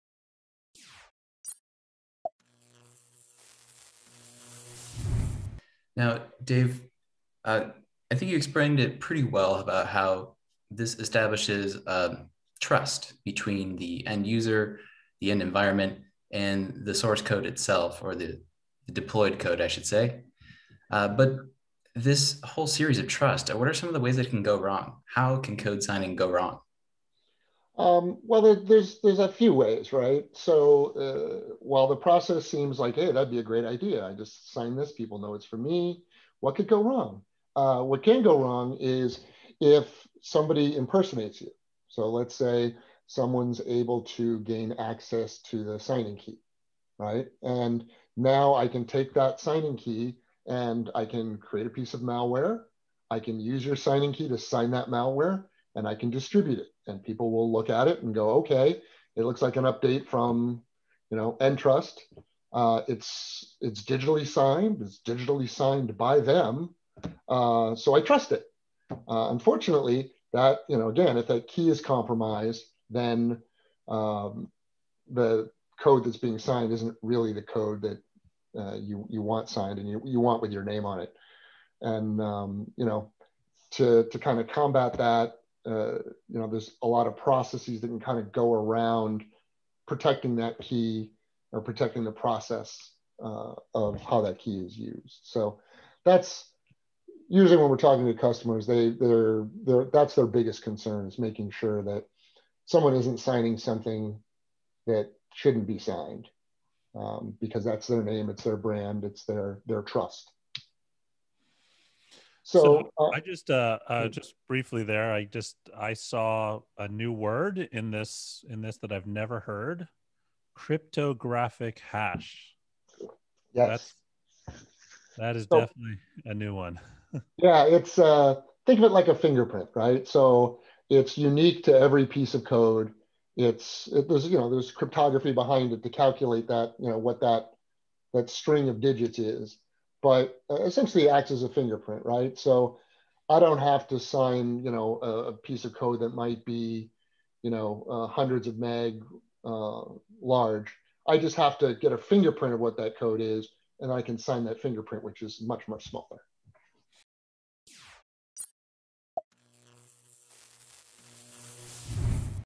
What are some ways that Code Signing can “go wrong”
ChannelBytes hosted an engaging virtual discussion with SynerComm and EnTrust, touching on topics ranging from “Code Signing” to “network attack surface” and “secure software development”.

This clip is from a larger discussion surrounding “Code Signing and Secure Development”.

In this clip we discuss how Code Signing can go wrong if someone impersonates you. Now people can take sign in keys to make a piece of malware, sign that malware and distribute it. If the key has been compromised, this isn’t something you want your name attached to.